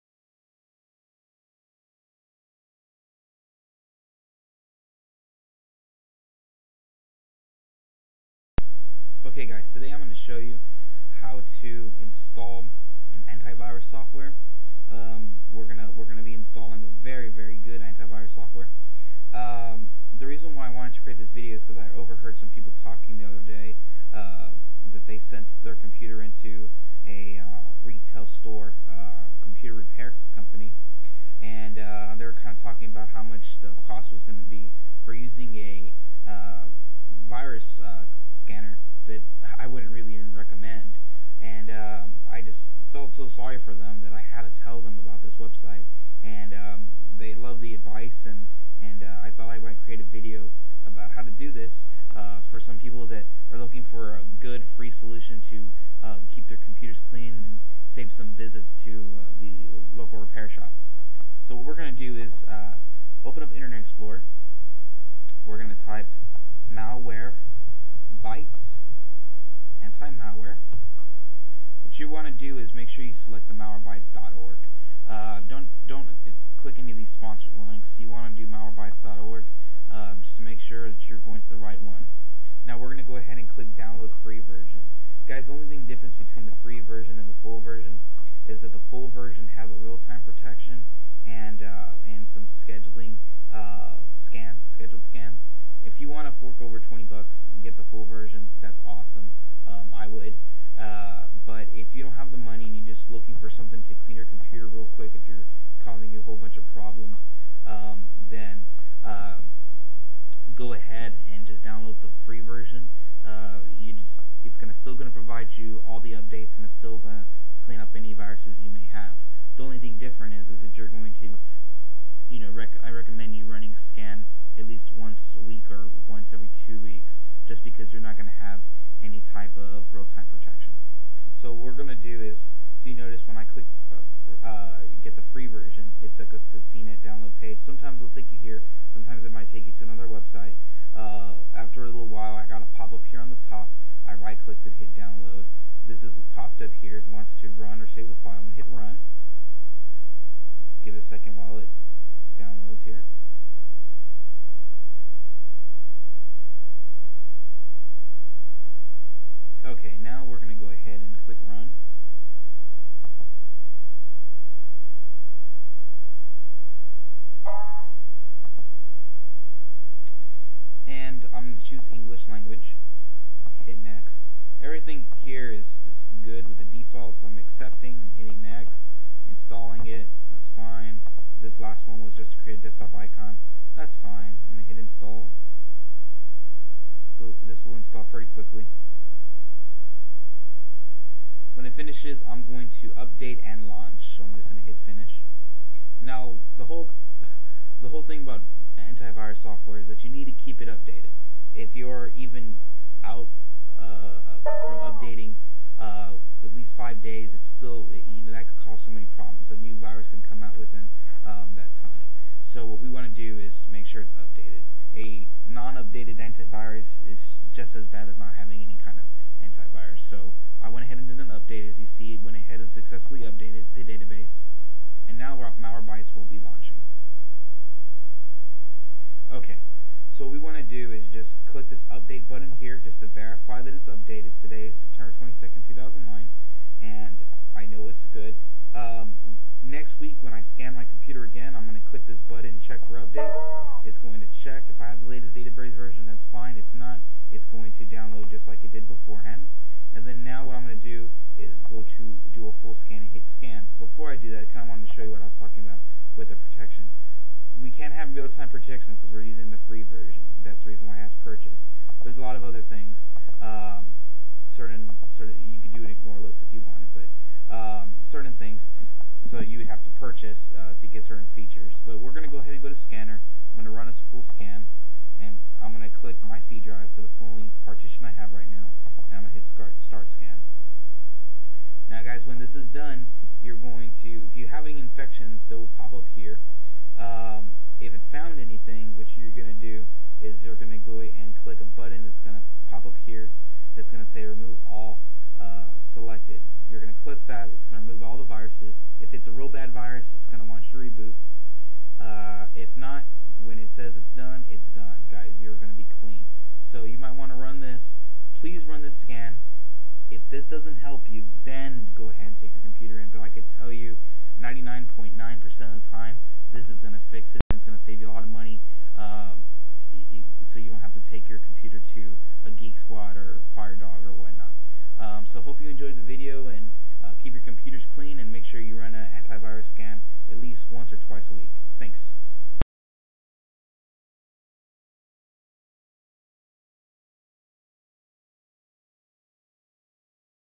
How to Remove A Computer Virus (Easy Tutorial)
Hello guys, im back again continuing my series of easy tutorials that have helped over 20,000 youtubers so far.I hope to impact peoples lives teaching them about computers and helping them change their financial future.

Today im going to show you how to remove a virus using Anti-Virus software, i will also be demonstrating and teaching you how to use the best FREE Anti-virus software